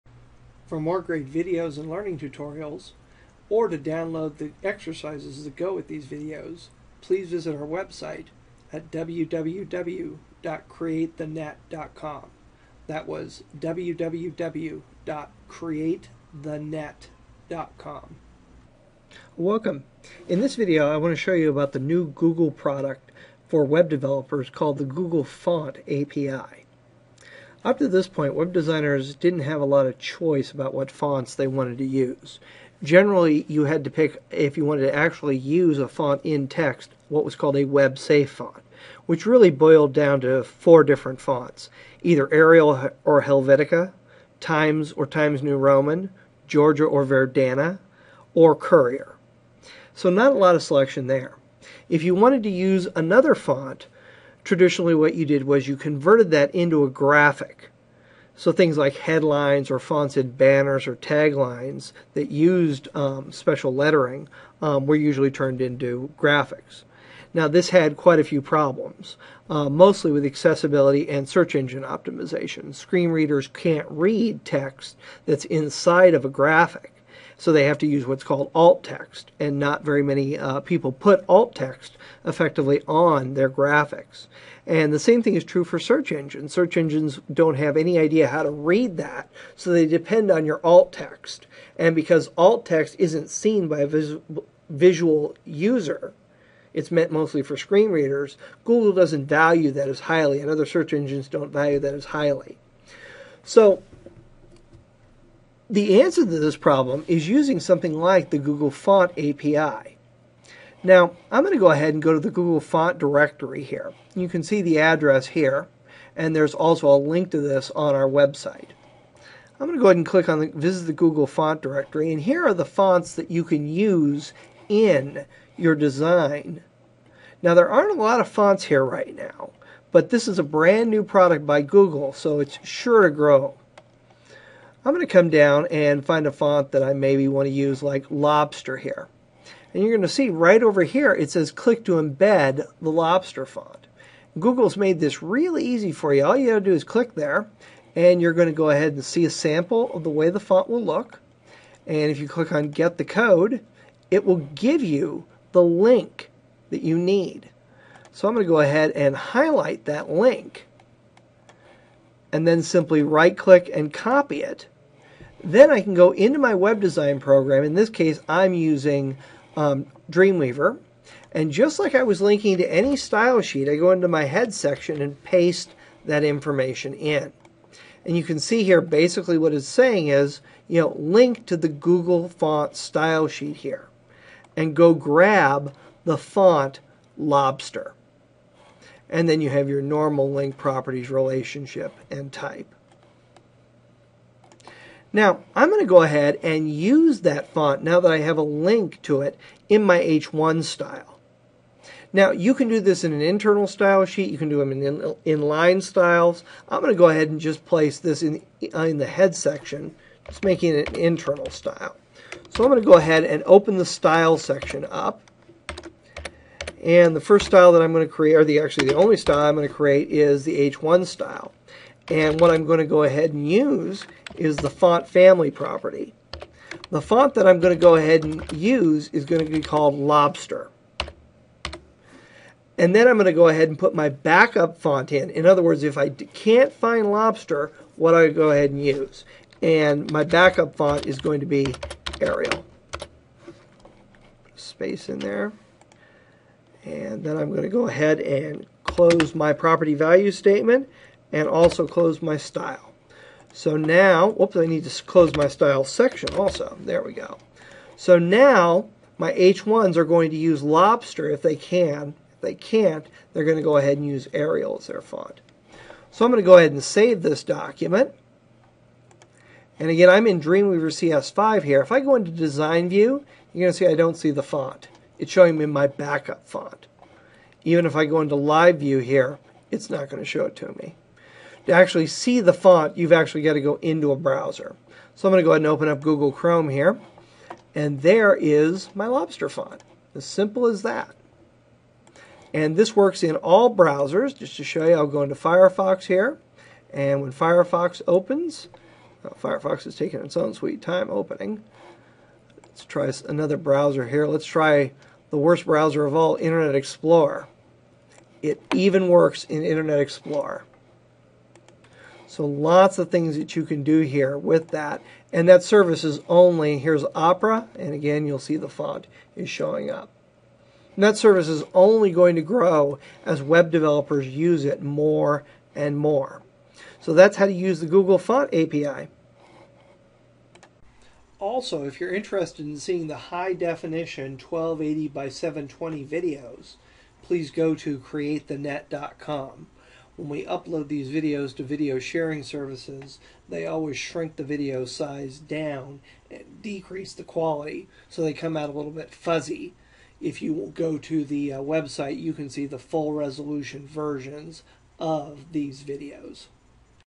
Using the Google Font API
Learn to use the Google Font API to use non-web standard fonts on your designs, without being concerned about the font being on the client computer.